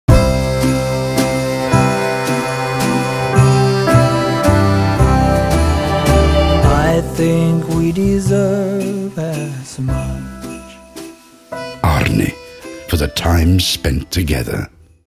Unboxing Vega - Ash
Aarni Vega Ash Watch is made of Finnish Ash Wood and equipped with a top-quality Swiss ETA movement.